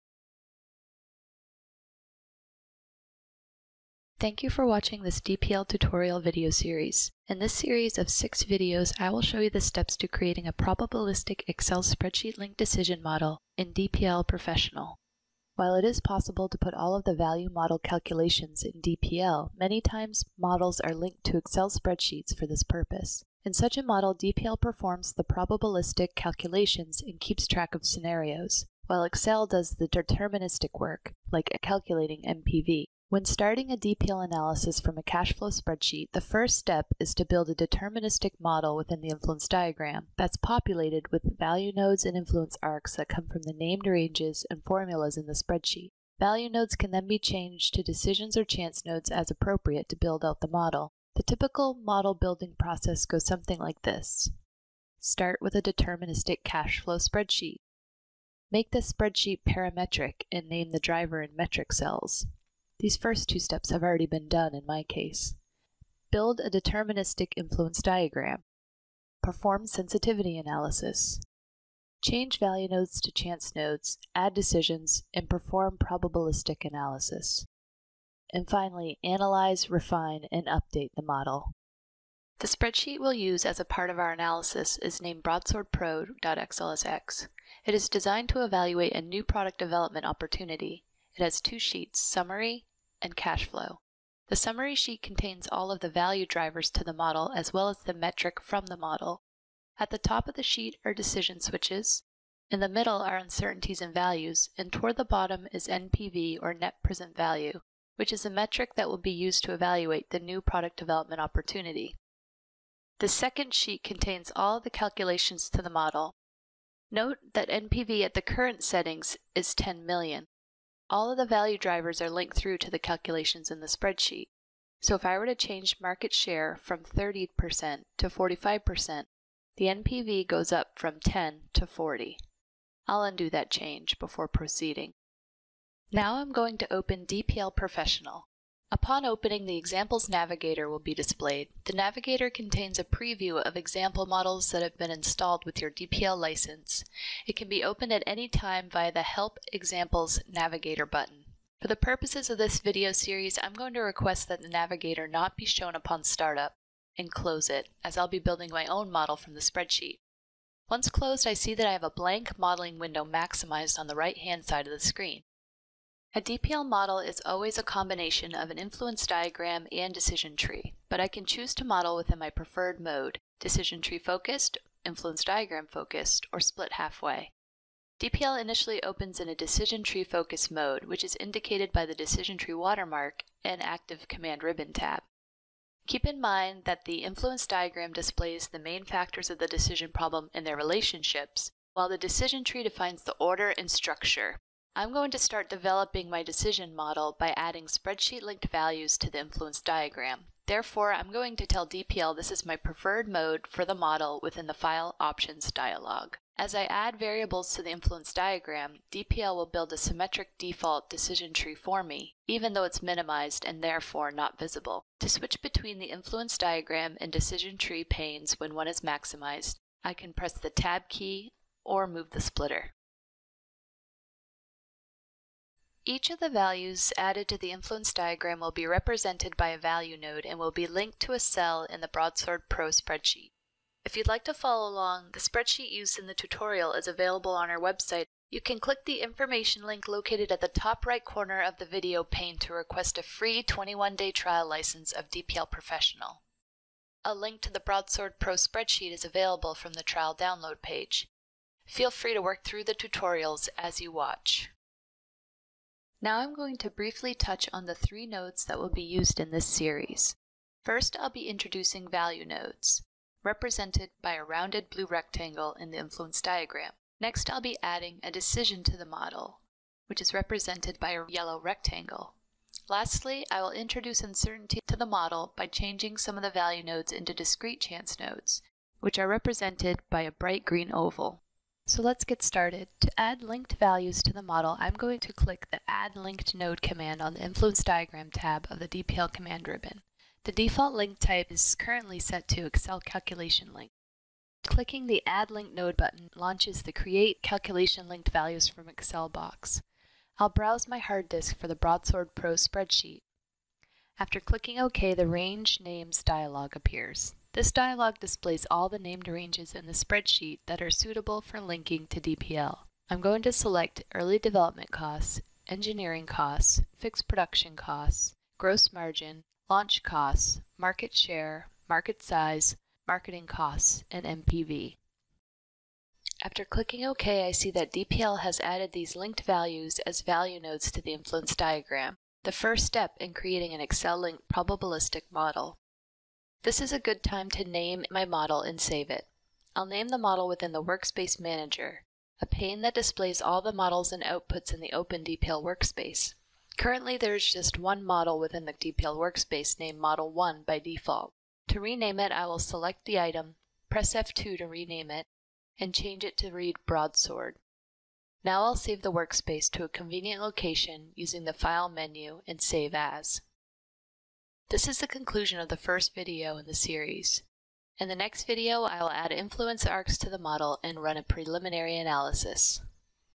DPL Tutorial Video #1: Creating a Deterministic Model from Excel
This is the first in a series of 6 videos of where I'll walk you through the steps to creating a probabilistic DPL decision analytic model from an Excel Cash Flow spreadsheet for a New Product Development opportunity. Within the first video I provide an overview of the spreadsheet and then quickly generate a deterministic model within DPL's Influence Diagram.

I would encourage you to build the decision model with me through out this series of tutorial videos. To do so, request a free 21-day trial license of DPL Professional: